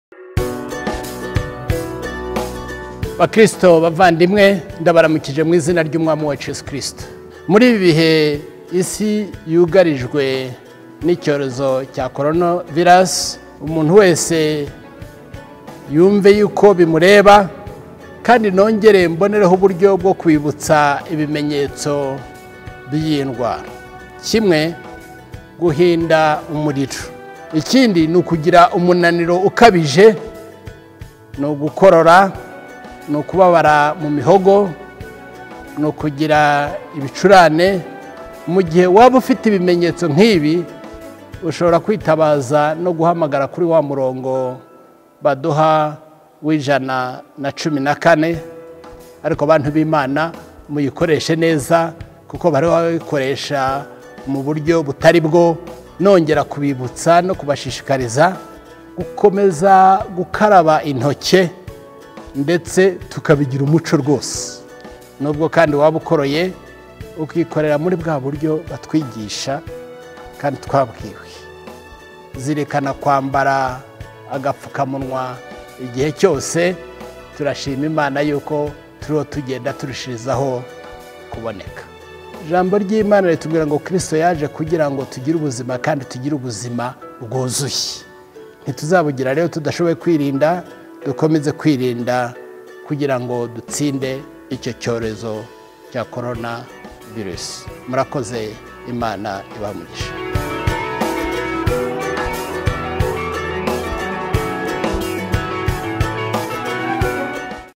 COVID-19: Ubutumwa bwa Arikiyepiskopi Laurent Mbanda (itorero Anglican) ku kwirinda COVID-19
Arkiyepisikopi Laurent mbanda arageza ubutumwa ku banyarwanda ku kwirinda COVID-19. Muri ubu butumwa Aratwibutsa ingamba zo kwirinda iki cyorezo, ibimenyetso by iki cyorezo, ndetse akanibutsa icyakorwa mu gihe hari ugize ibyo bimenyetso.